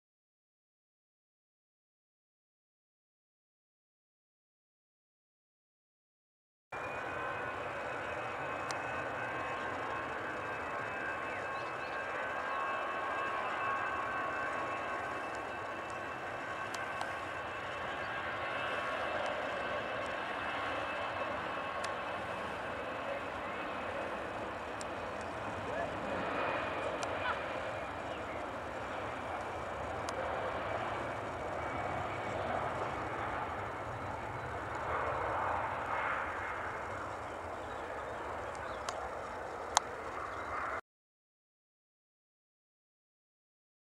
Easyjet A320 G-EZTY On U21835 Takeing Off At Manchester Airport ON Runway 05L 02/05/2014
Easyjet Airbus A320-214 G-EZTY On Flight U21835 To (AMS) Amsterdam Takeing Off At Manchester Airport ON Runway 05L On The 02/05/2014